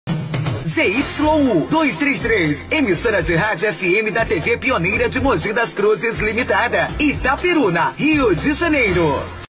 Antigo Prefixo - Jovem Pan FM - 91,3 MHz - Itaperuna/RJ
Antiga vinheta de prefixo da Rádio Jovem Pan FM, 91,3 MHz, de Itaperuna, interior do Rio de Janeiro
ZYU 233.
Slogan: A número um do Brasil.
Emissora voltada ao público jovem, que reproduz musicas em sua maioria internacionais.
Pertence à TV Pioneira de Mogi das Cruzes Ltda.

Atualmente uma nova vinheta de prefixo é veiculada.